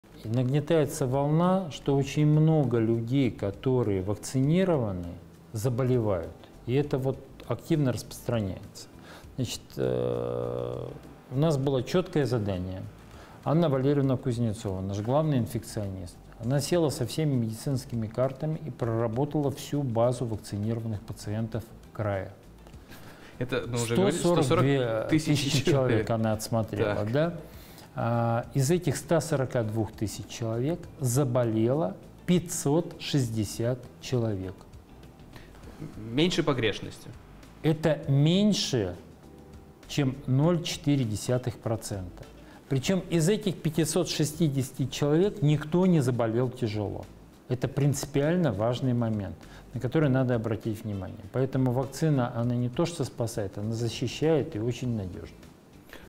Только 0,4 % привитых от коронавируса в Хабаровском крае заболели ковидом
Сейчас нагнетается волна относительно того, что очень много людей, которые вакцинированы, заболевают. Зампред правительства Хабаровского края Евгений Никонов опроверг это мнение на примере нашего региона.